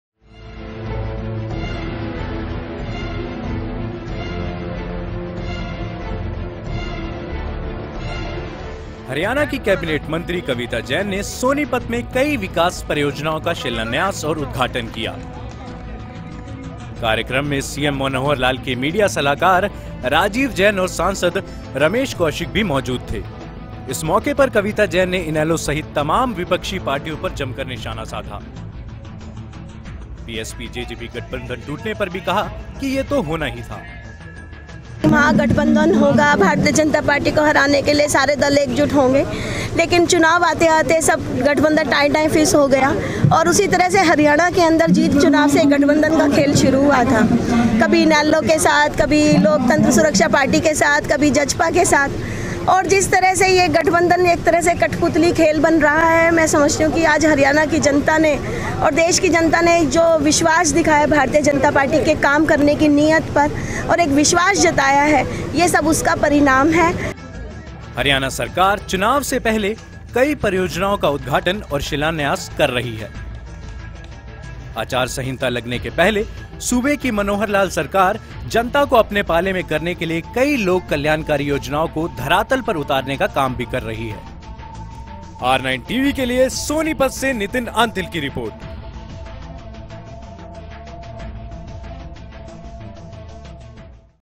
SONIPAT को कई सौगात
सोनीपत में हरियाणा की कैबिनेट मंत्री कविता जैन ने कई विकास परियोजनाओं का उद्घाटन किया...